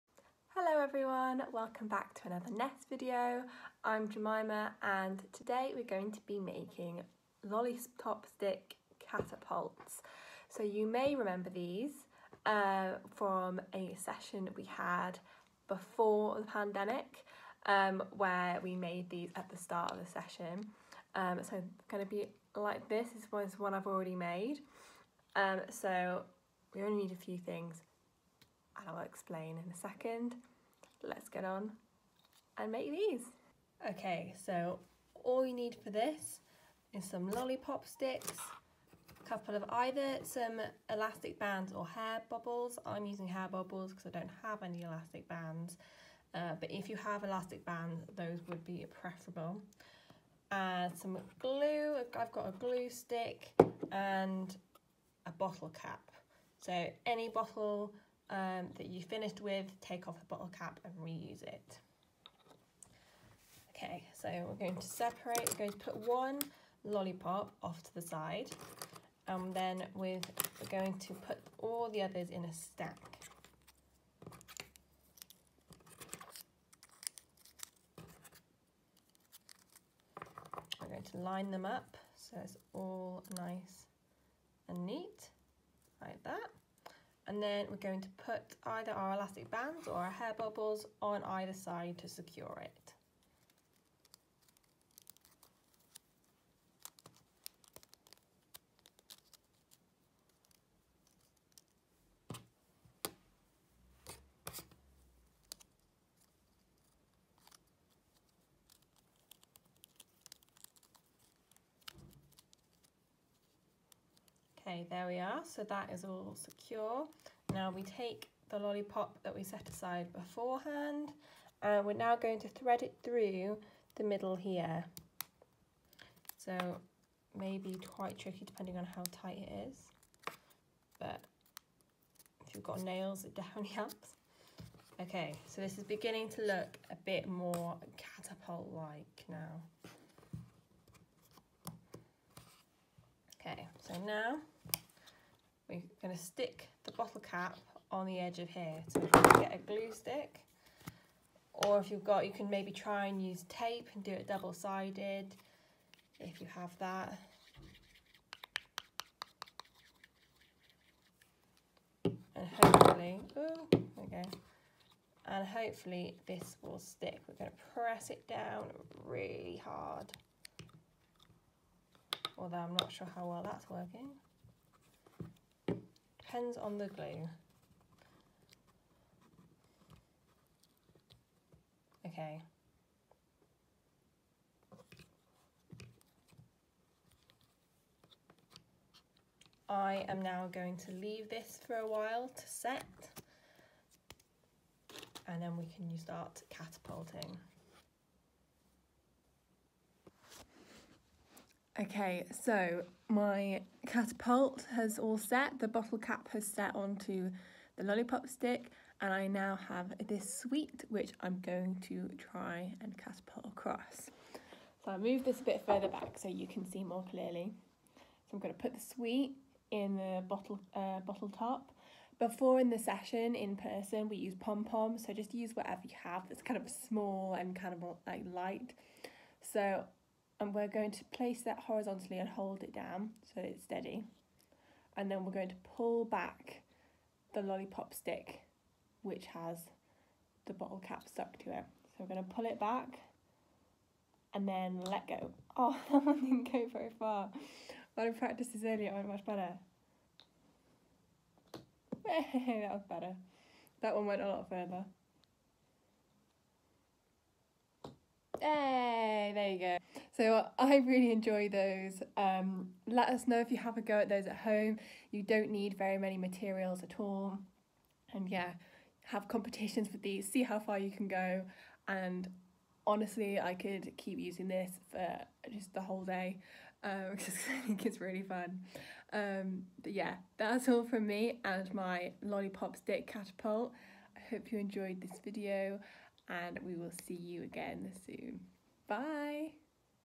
Making Lollipop Catapults! | N.E.S.T Kids 19/09/2020
Jemima shows us how to make catapults out of lollipop sticks, bottle caps and hair bobbles!